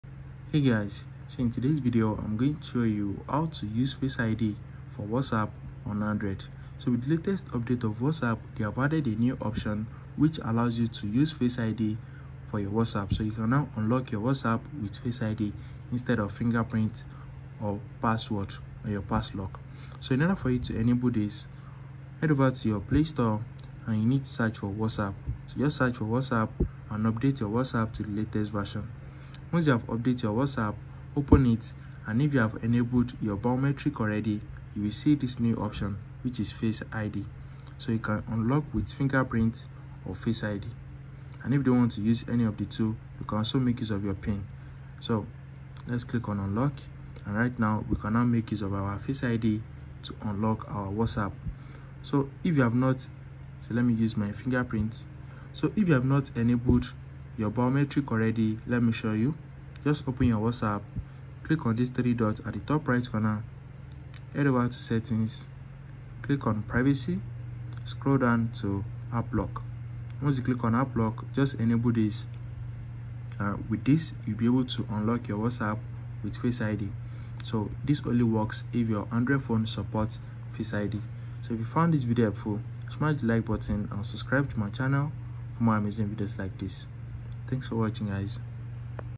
How to Use FaceID for WhatsApp on Android - Quick and Easy Guide!
Learn how to easily use Face ID for WhatsApp on your Android device with our comprehensive guide! Whether you're concerned about security or seeking a convenient way to access your messages, this tutorial has you covered. Discover simple methods to set up and utilize Face ID for WhatsApp on your Android phone. Watch now and enjoy seamless and secure access to your WhatsApp messages with Face ID!